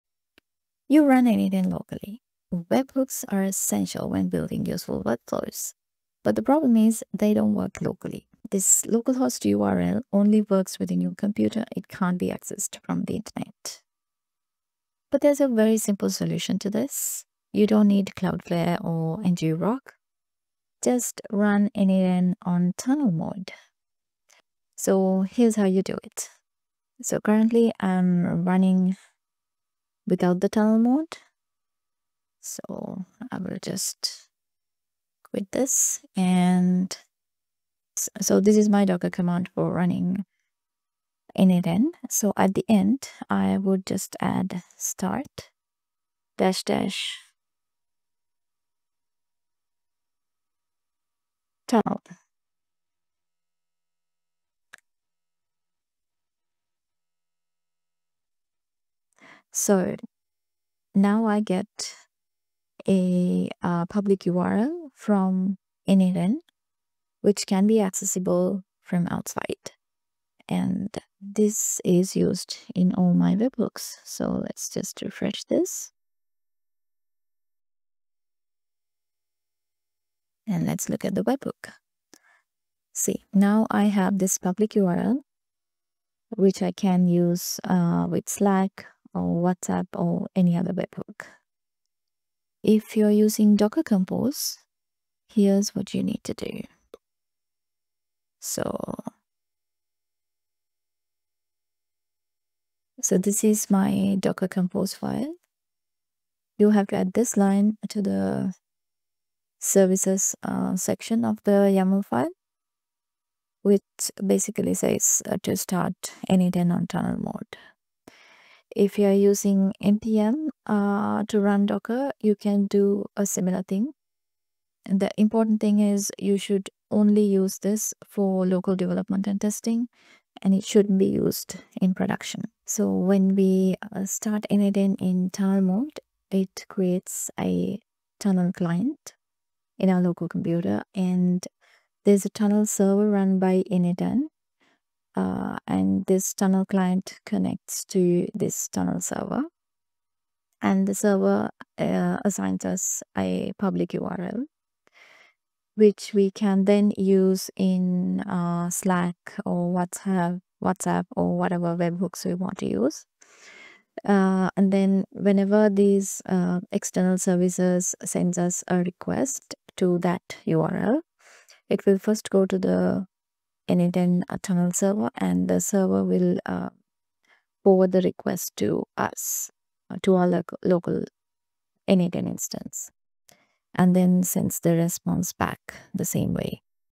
Use webhooks locally in n8n
You can easily test n8n webhooks on your local environment by running n8n in tunnel mode.

This will give you a webhook url that is accessible outside your computer.

It's very simple,

start n8n with the option --tunnel

If using npm or docker run command, simply add --tunnel at the end of the run command

eg:

docker users: docker run -it --rm \ --name n8n \ -p 5678:5678 \ -v n8n_data:/home/node/.n8n \ \ start --tunnel

for npm users: n8n start --tunnel

If you are using docker compose:

add the following line in the docker compose file.

command: ["start", "--tunnel"]

I find it very convenient that,
Once you run in tunnel mode, all your existing webhooks will be switched to the new tunnel url, and when you come out of it, it will go back to the localhost url.

What I have noticed is that the webhook url remains same even if you restart the n8n instance.